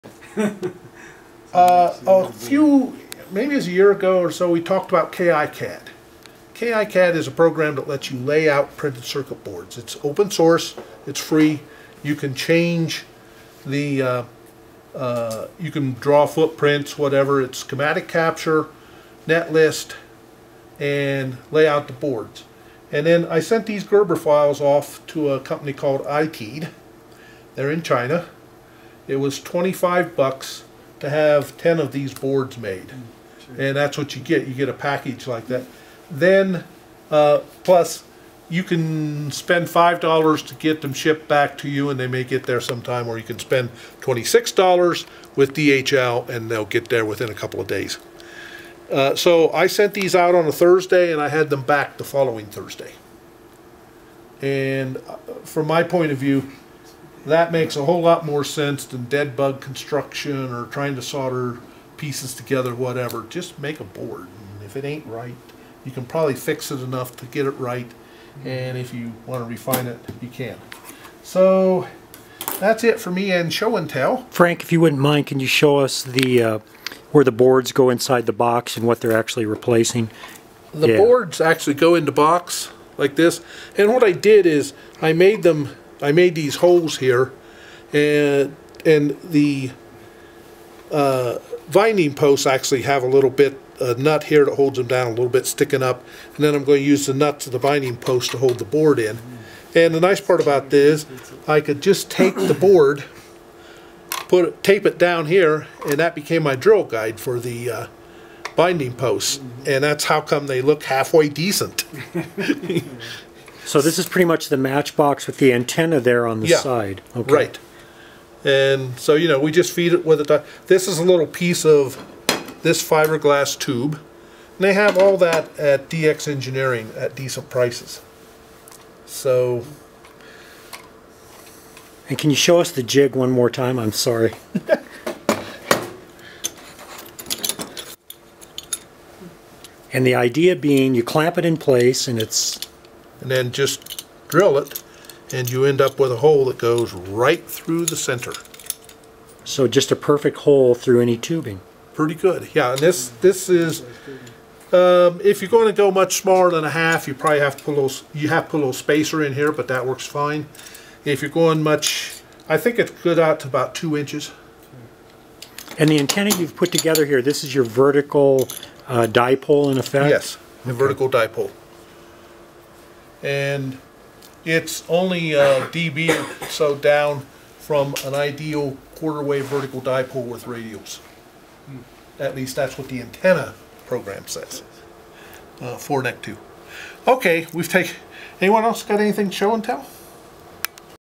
KiCad demonstration by K0FEI also includes his vertical dipole and tube drilling jig
KiCad is an open source software suite for electronic design automation. CQC President Frank Ivan, K0FEI, used KiCad and other resources to begin construction on the match box for his home-brewed vertical dipole. Also in this video is a brief explanation of the tube drilling jig he used for this and other projects.

This recording was made by Roger J. Wendell, WB0JNR, during the Colorado QRP Club's May 11, 2013 general meeting.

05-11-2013